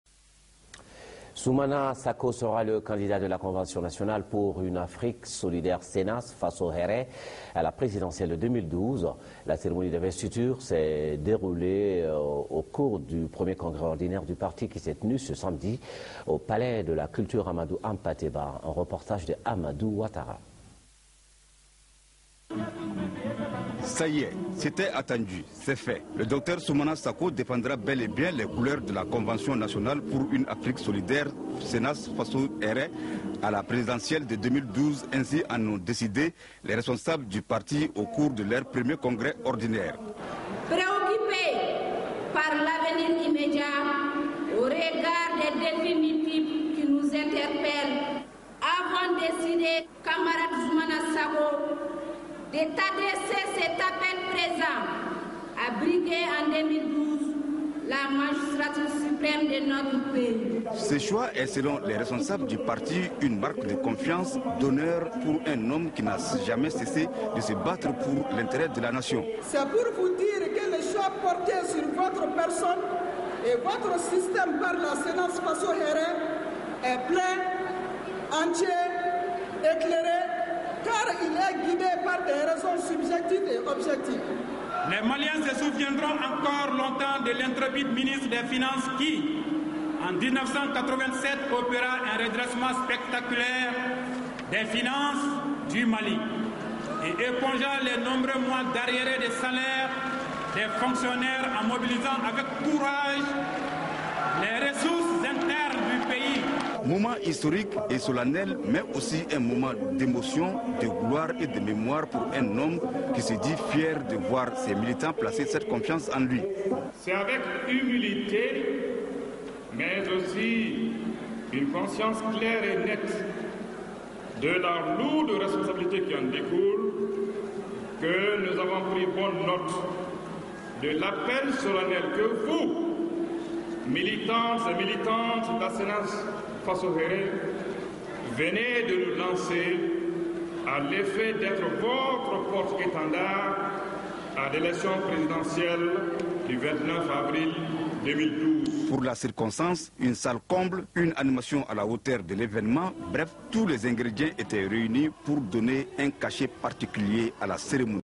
Investiture de soumana sako comme candidat du parti à la présidentielle de 2012
Investiture de soumana sako comme candidat du parti à la présidentielle de 2012 lors du premier Congrès Nationale de la CNAS-Faso Hèrè